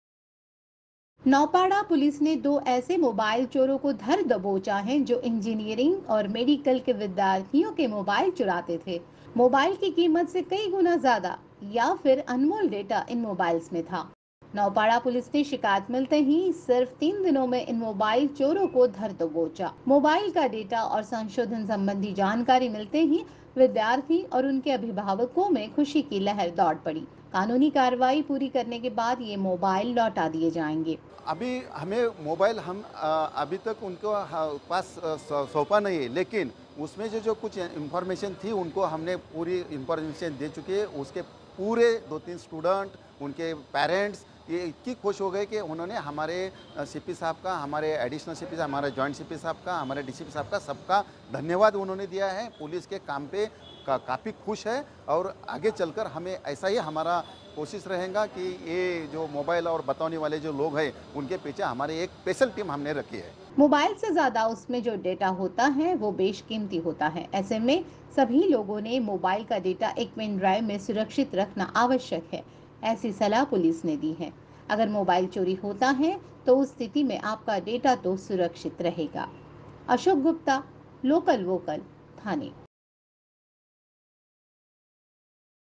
Mobile Thief Arrested I Thane Police I केवल तीन दिन में मोबाईल चोर गिरफ्तार I Mobile Data
Naupada police from Thane have arrested two mobile thieves who used to steal mobiles from Engineering and Medical students. After a complaint by an engineering student who stored his research papers in his mobile, which was stolen by the thieves, police swung into action and nabbed two thieves and returned the invaluable data to the student.